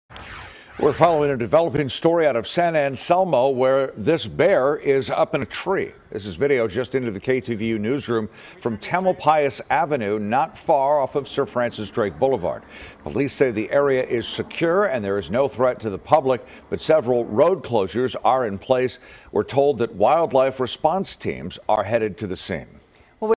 Bear climbs backyard tree in San Anselmo, briefly causes shelter-in-place
A bear that climbed a tree in a backyard, causing a shelter-in-place in San Anselmo Thursday, came down on its own and was headed for open space, officials say.